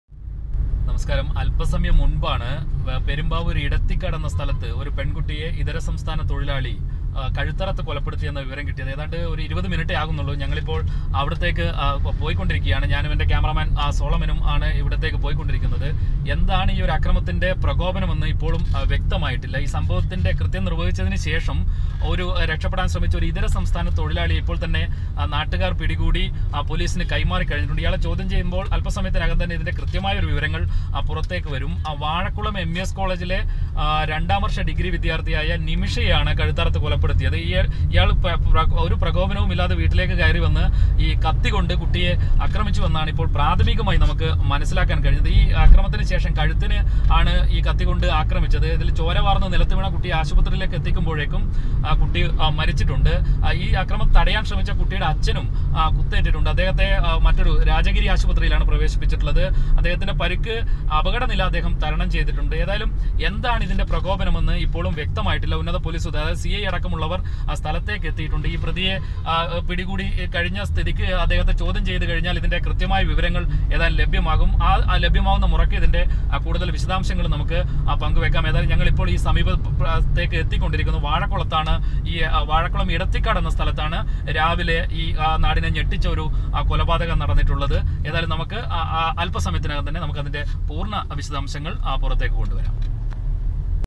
22-year old girl hacked to death in Perumbavoor
22-year old girl hacked to death in Perumbavoor